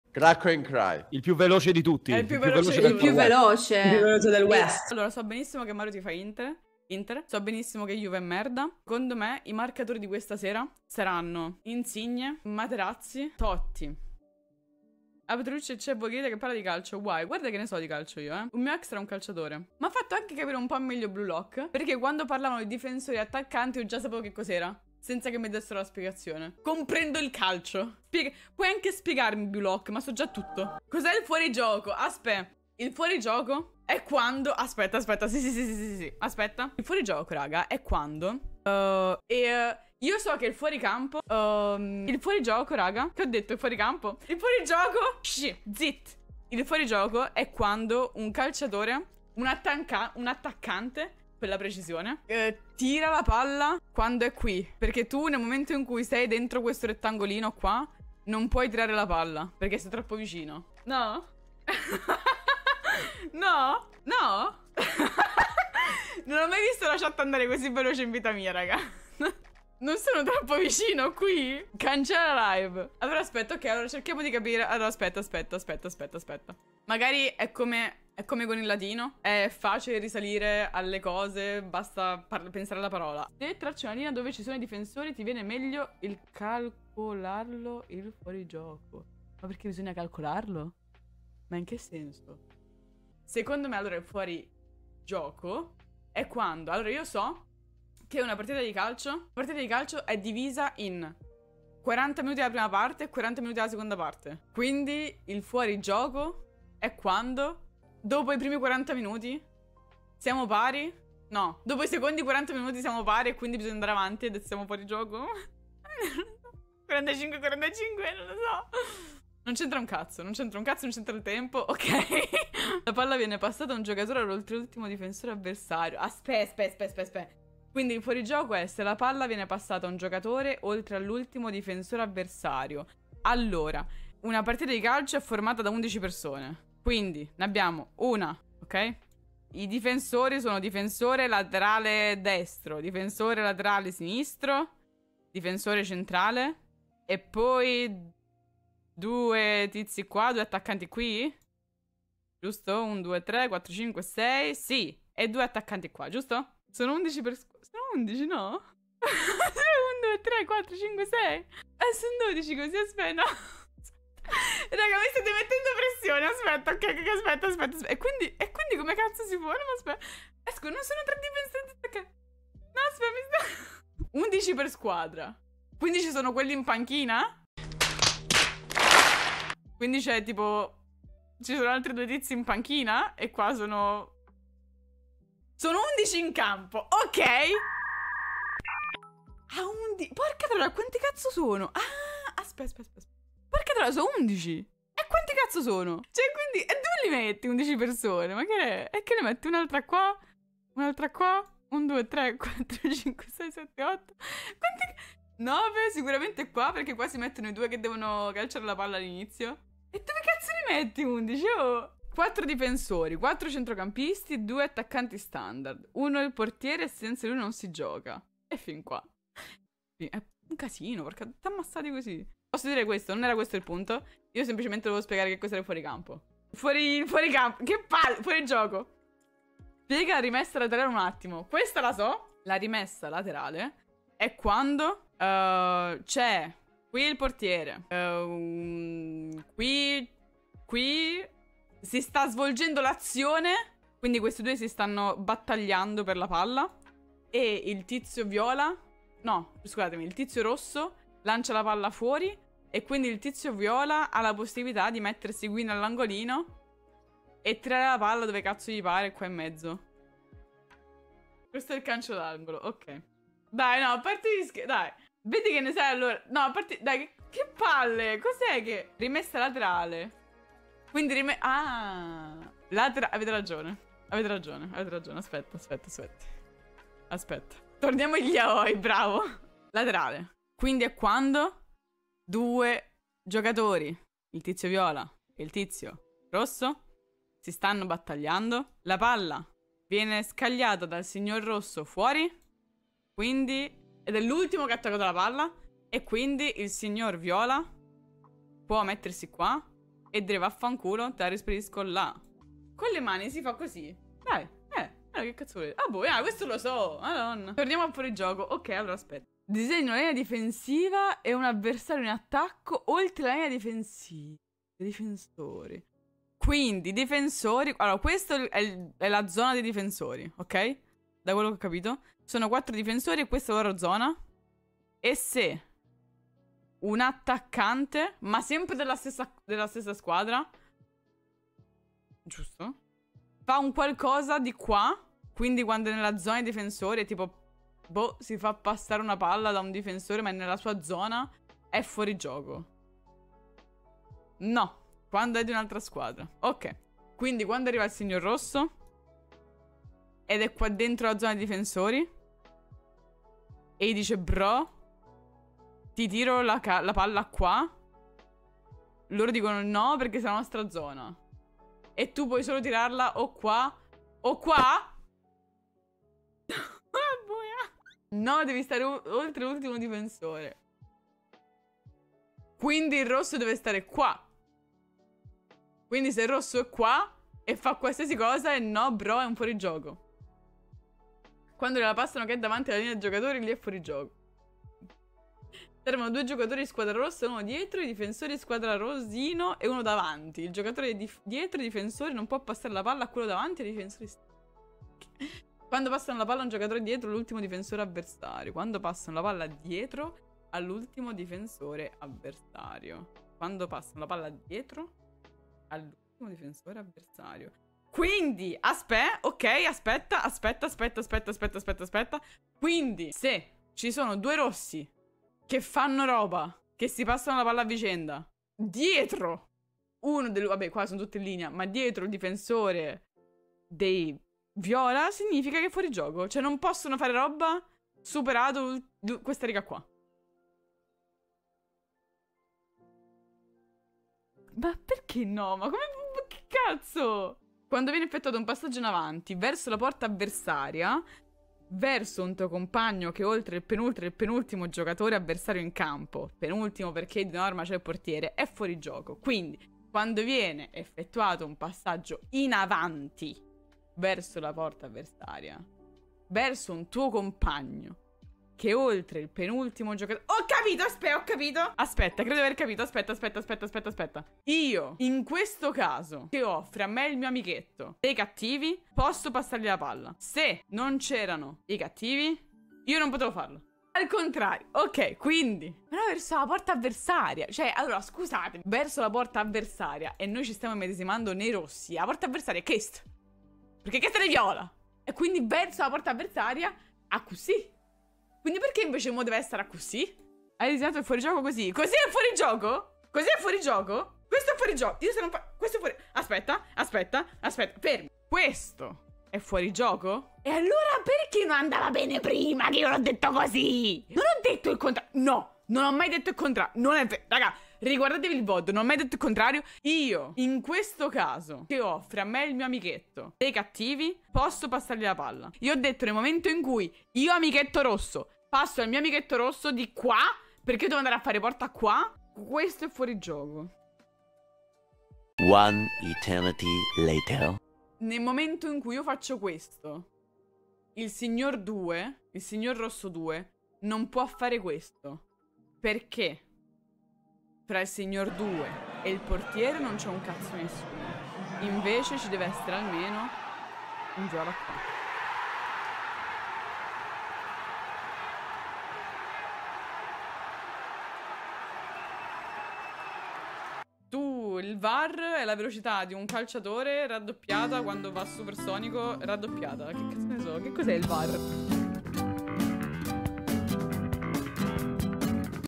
VOGHELITA CERCA DI CAPIRE COS'E' IL FUORIGIOCO
Se siete presenti nel video e volete che venga rimosso scrivetemi su Instagram
0:00 Intro
0:05 Conoscenza infinita
0:31 Arriva La domanda
2:36 Quanti calciatori giocano?
3:45 Rimessa laterrale
5:08 Torna al fuorigioco
8:12 Niente da fare
8:44 La volta buona?
9:14 Prossimo episodio?